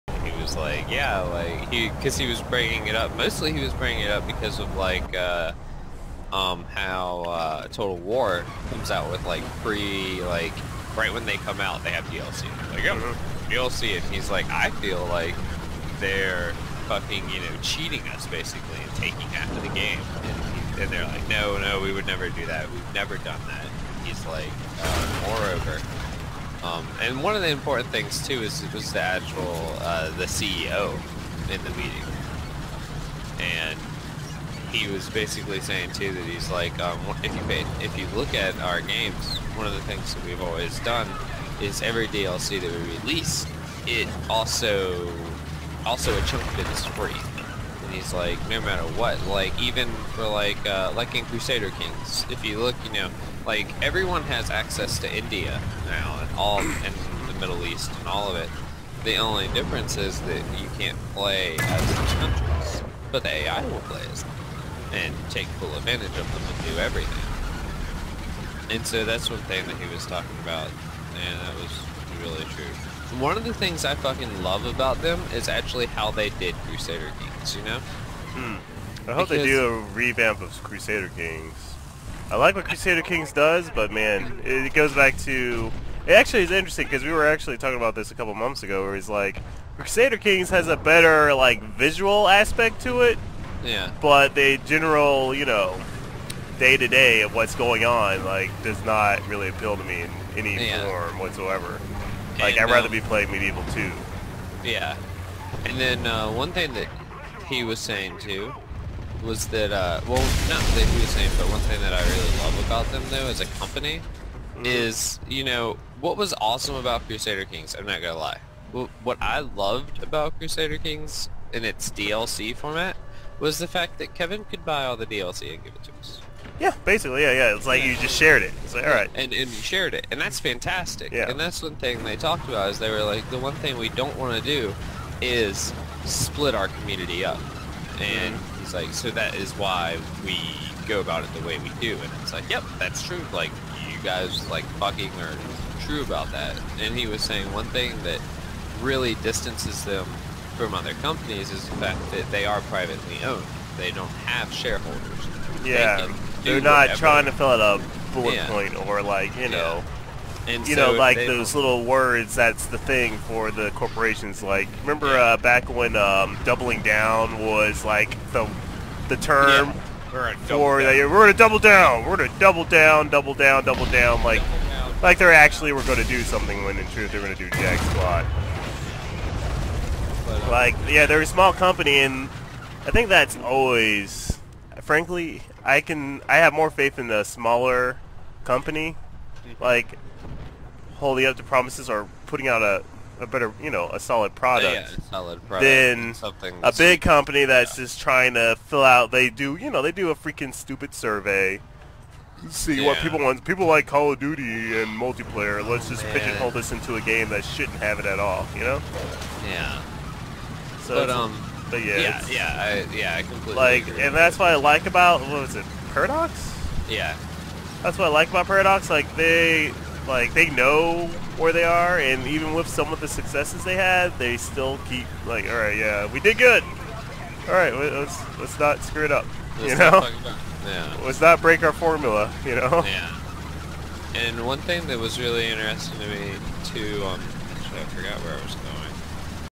Video Game Hype Check: Paradox & dlc
A small Candid clip with me and a friend talking about Paradox interactive and DLC.